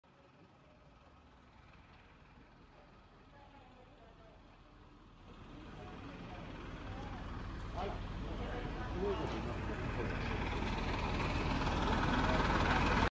【火事】都内某所で大火災、なす術なく立ち尽くす消防隊員。
火はプライバシーの関係で載せておりません😀